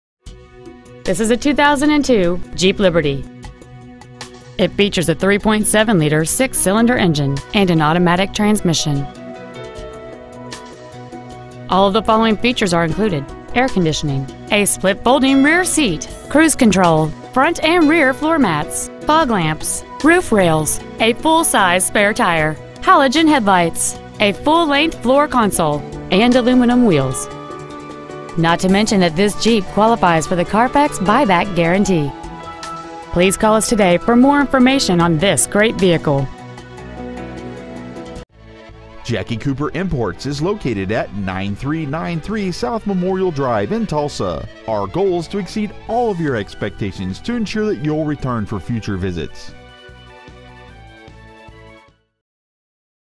Pre-Owned 2002 Jeep Liberty Tulsa OK 74133
Year : 2002
 Make : Jeep
 Model : Liberty
 Engine : 3.7L DOHC V6 Power Tech engine
 Trans . : Automatic
 Exterior : Black
 Miles : 134,551
 Interior : Other
 Stock : 2W357294